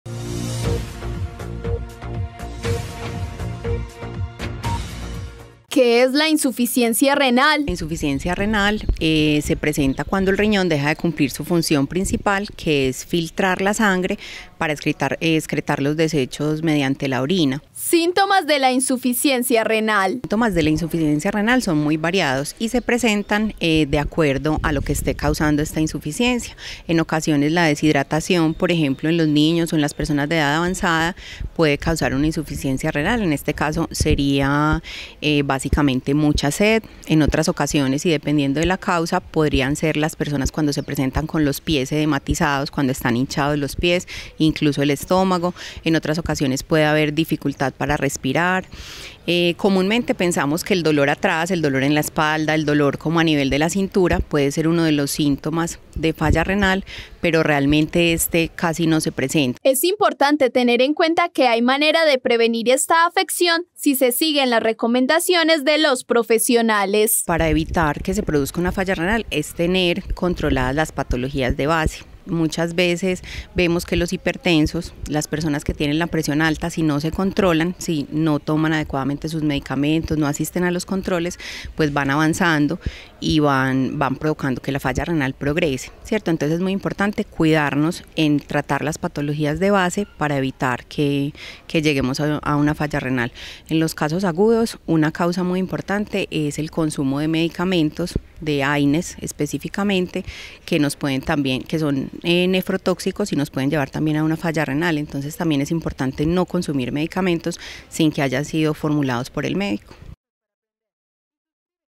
La insuficiencia renal
La insuficiencia renal es la afección que provoca que los riñones pierdan la capacidad de eliminar los desechos y equilibrar los fluidos. La doctora Eliana Jiménez nos tiene más información al respecto.